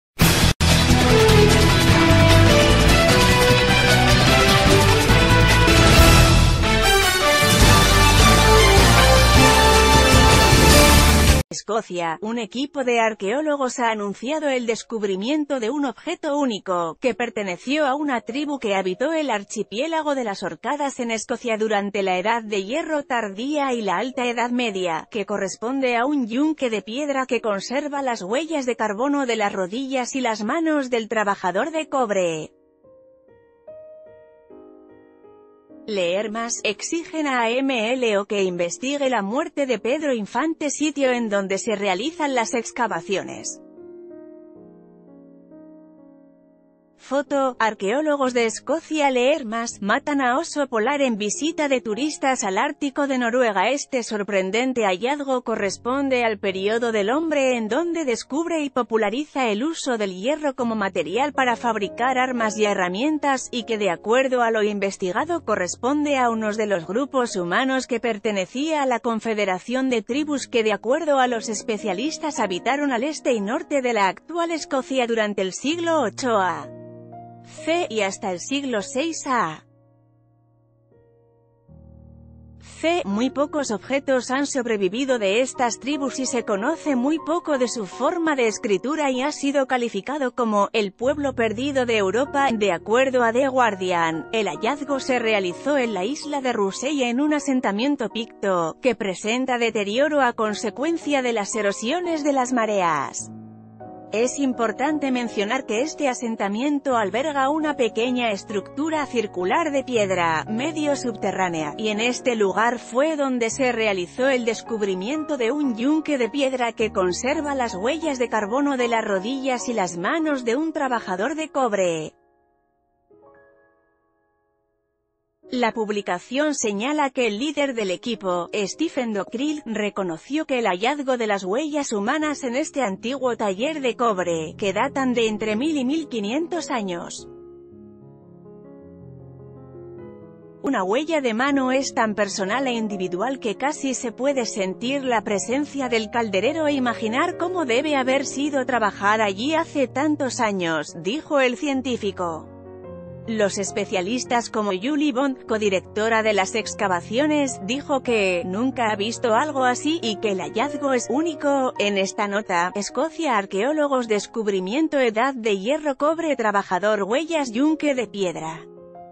Hallan yunque de piedra de un taller de la Edad de Hierro
Escocia.- Un equipo de arqueólogos a anunciado el descubrimiento de un objeto único, que perteneció a una tribu que habitó el archipiélago de las Orcadas en Escocia durante la Edad de Hierro Tardía y la alta Edad Media, que corresponde a un yunque de piedra que conserva las huellas de carbono de las...